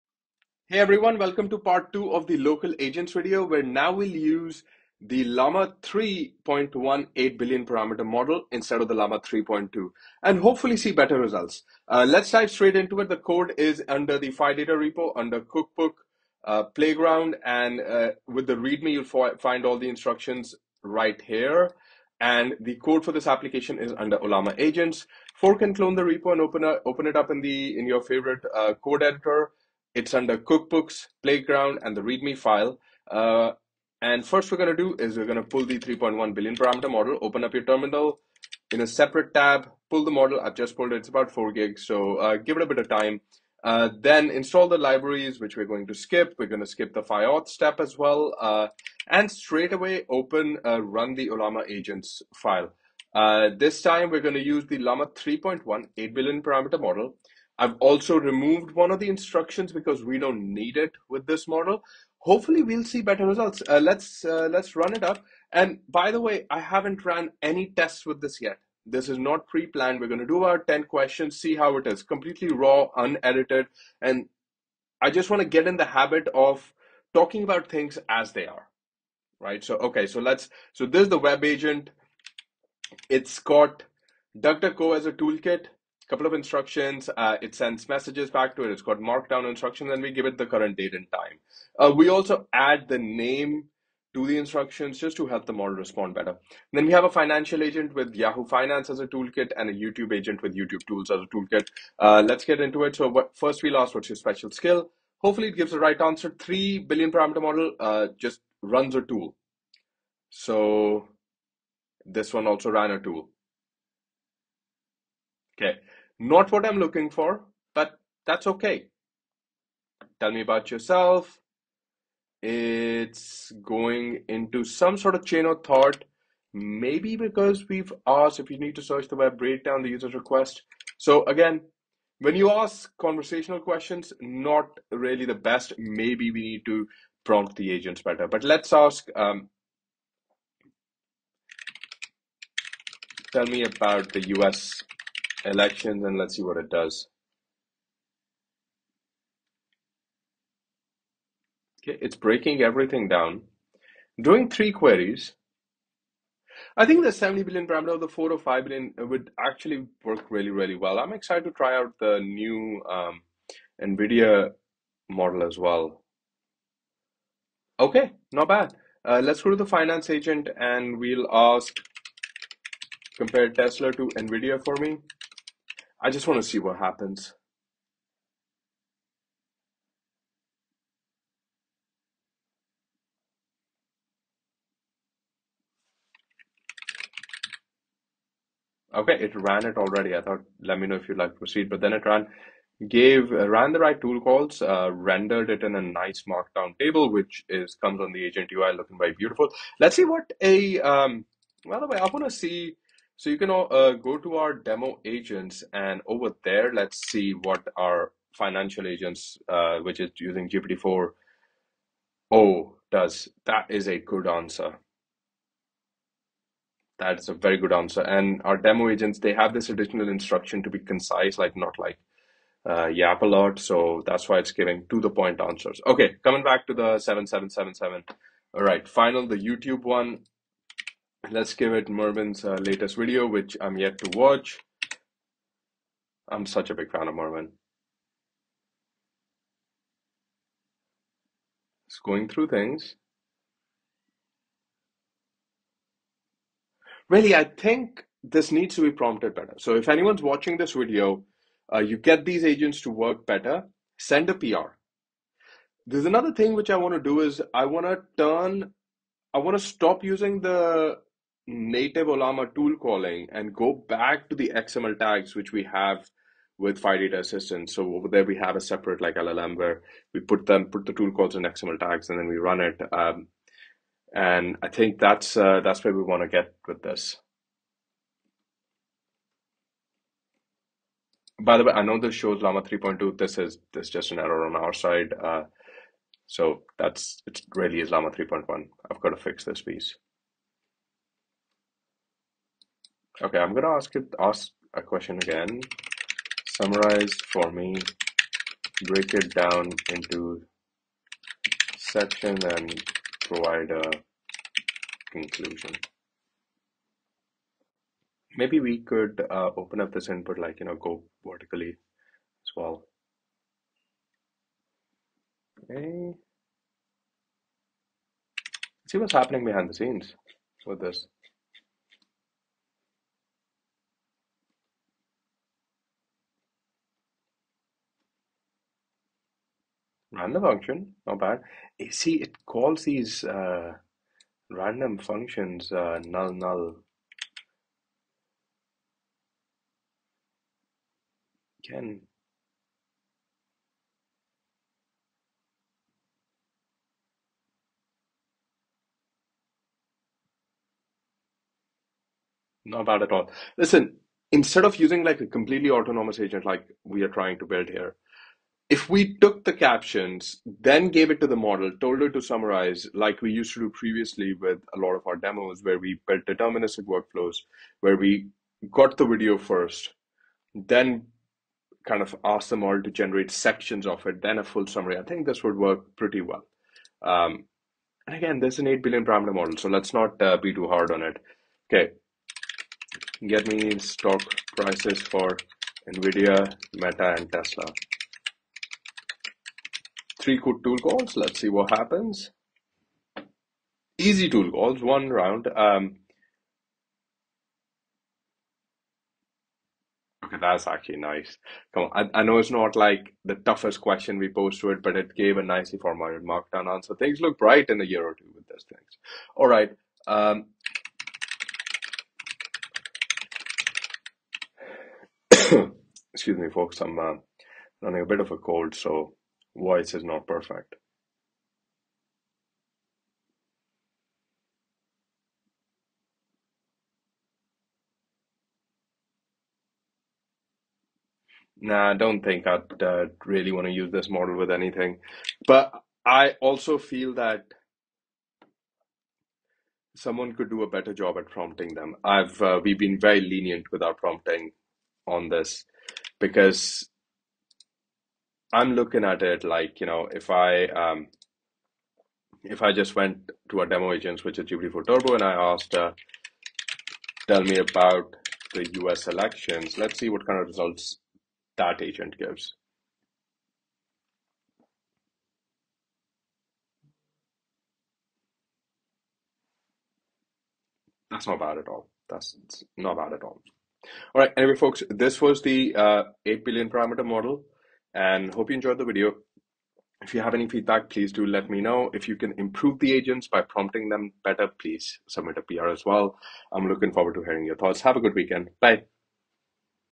Local Agents with Llama 3.1:8b ✨ | Phidata
This is Part 2 of the Fully local Agents with Ollama + Agent UI ✨

🏆 Pros: local, private and free 🫡
⚠️ Cons: works 50% of the time 🤷‍♂️